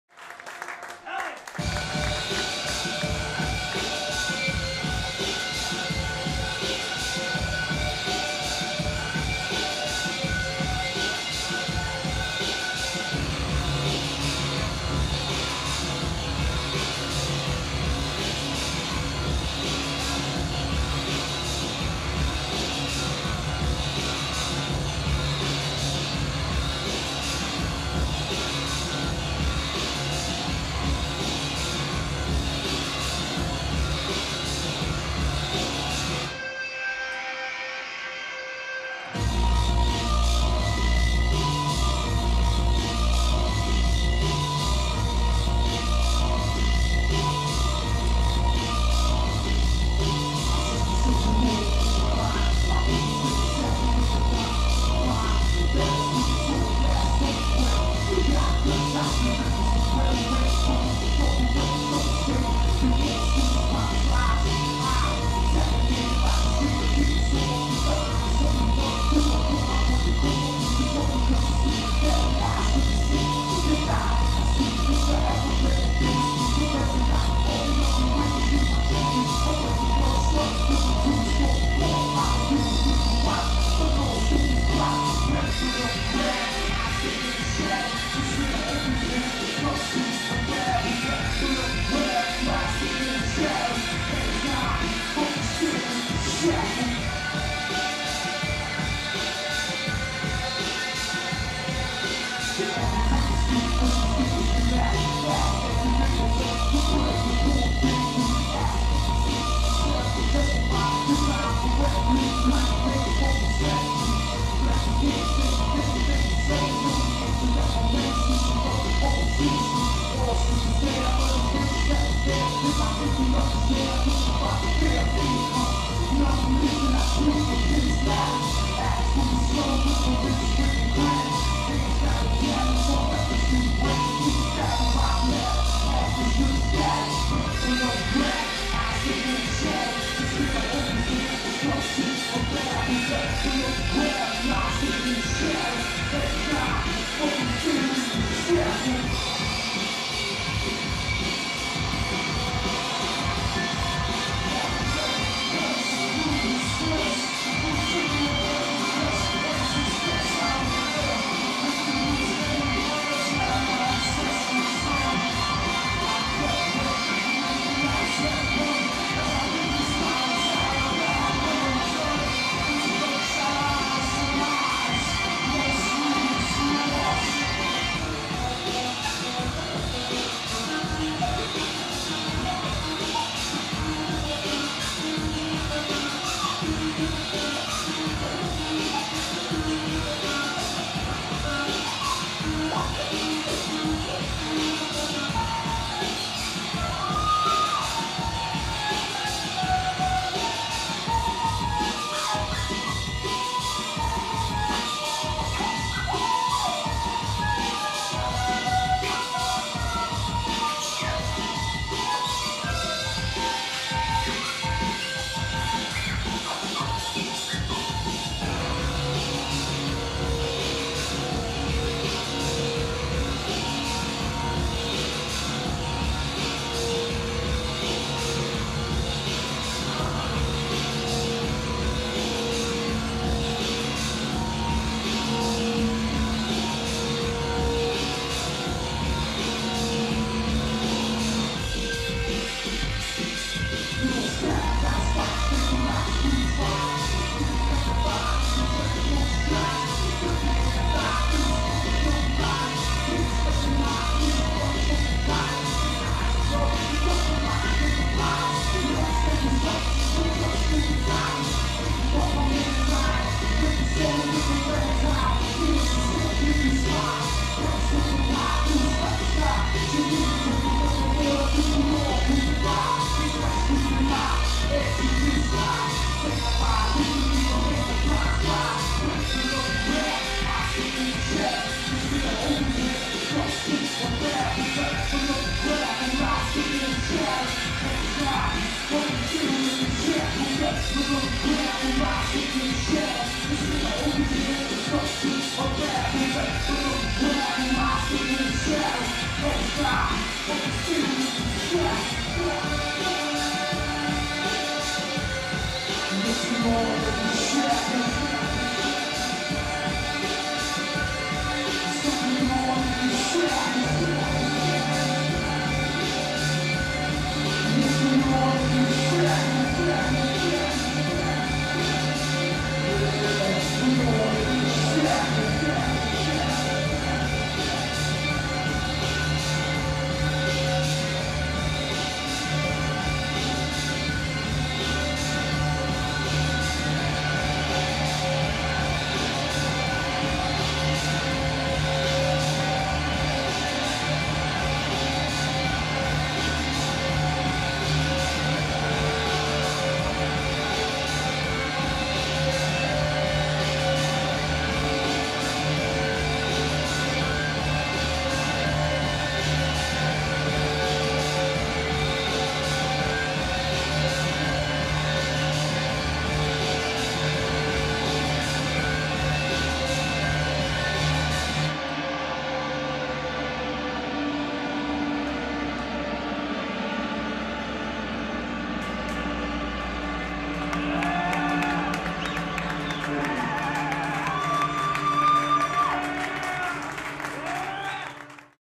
dalek @ Louie's 7.12.16 no5
Dalek performing live at Louie's Trophy House Grill in Kalamazoo, Michigan, 7.12.16.